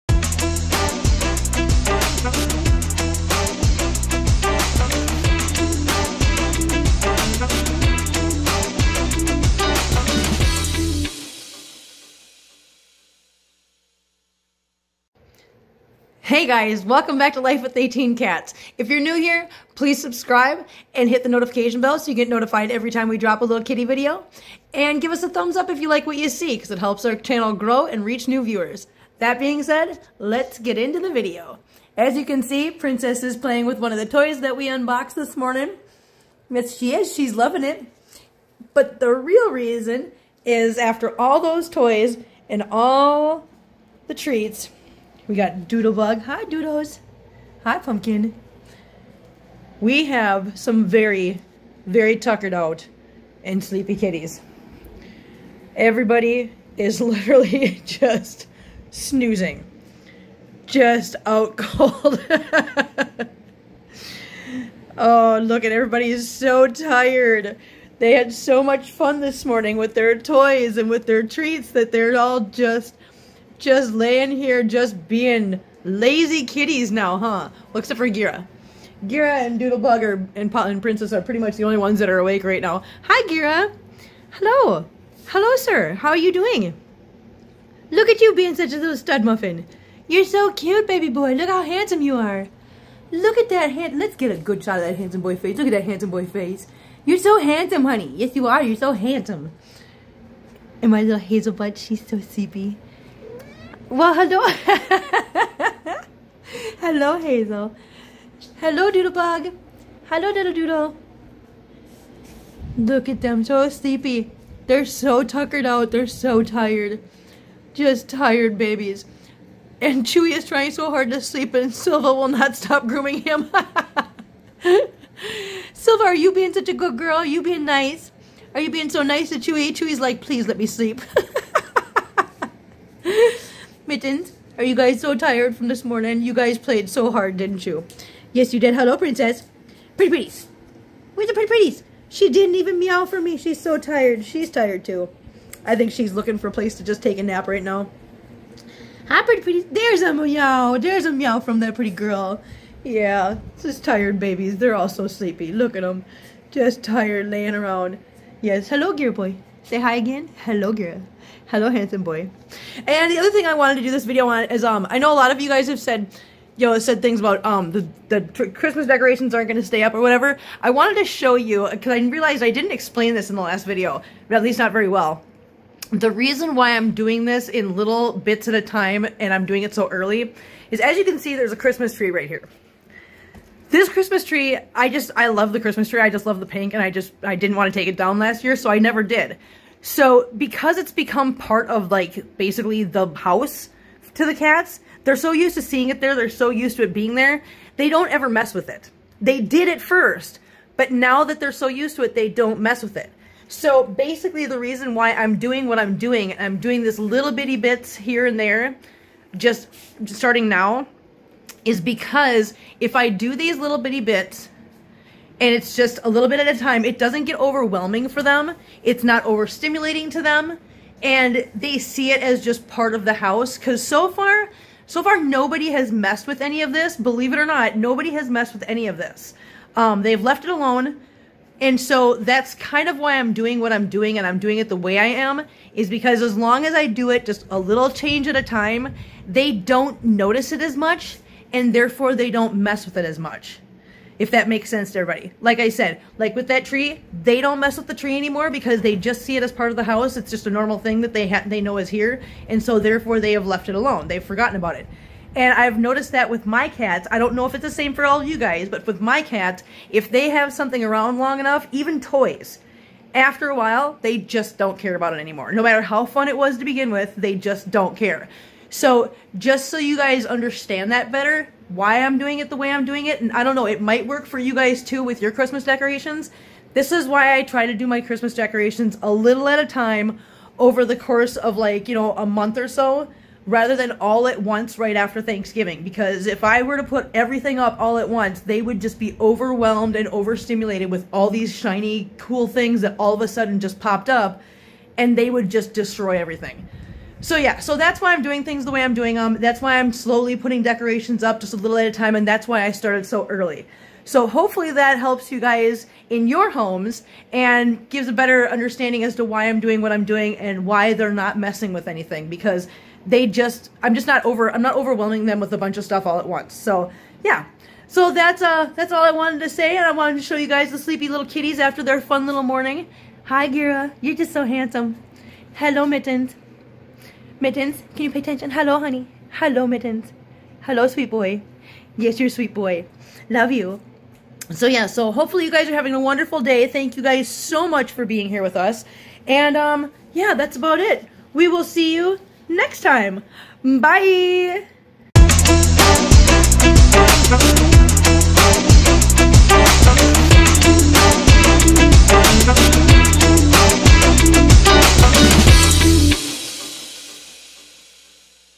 Cat Nap and Holiday Decorations | Life With 18 Cats
This cat family is tuckered out and piling up for the lazy cay nap kind of day! The sleepy cats give Kim the chance to talk with us about decorating for the holidays with a full house of cats! Catch a glimpse of Kim's DIY Cat Stockings and display for the cats Christmas treats!

This is the best way to help our channel grow right meow!